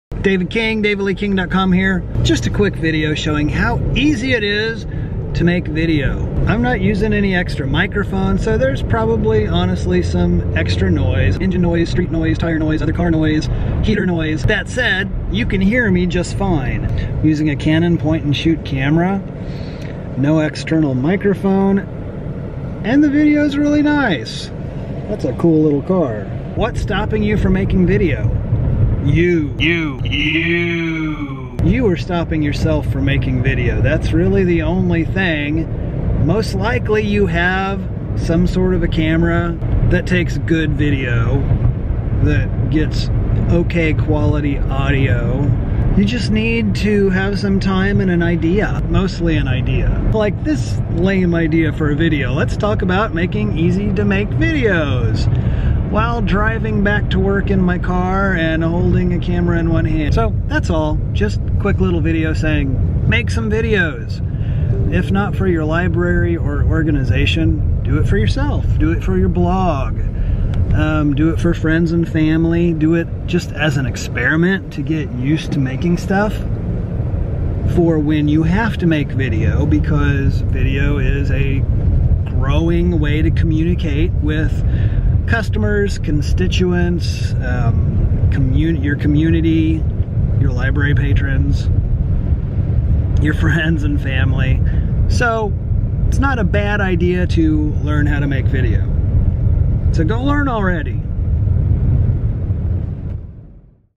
Making Video is Easy
Just a short video demonstrating how easy it is to make videos! You should be making some, so start already!

In this video, I use a Canon G7X Mark II point and shoot camera - to demonstrate how easy videos are to make and share.